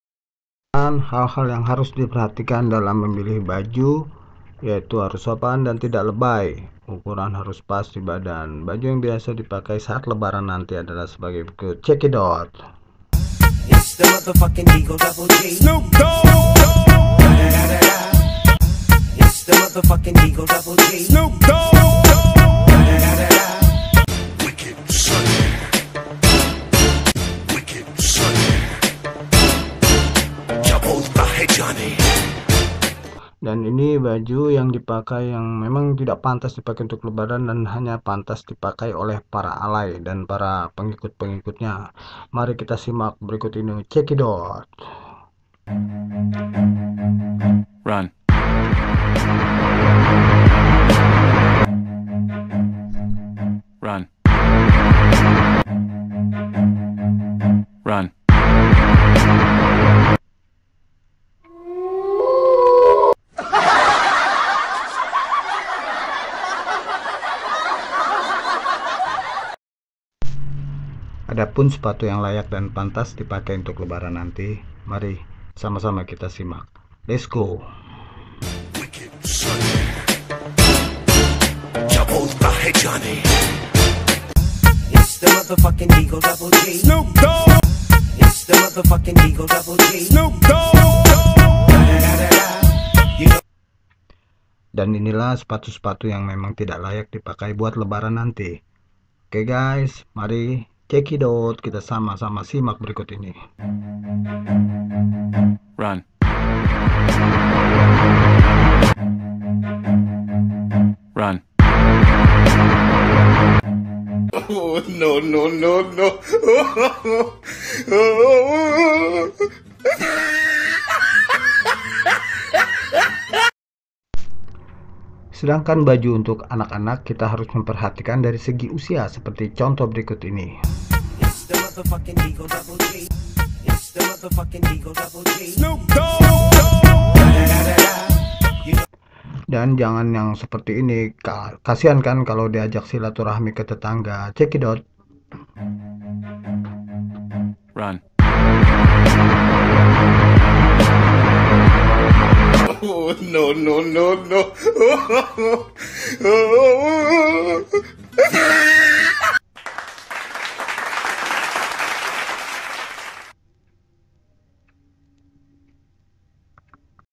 Tips dan cara memilih baju untuk lebaran
Lebaran identik dengan pakaian, sepatu dan celana ini adalah pakaian yang baik dan kurang baik dipakai berlebaran nanti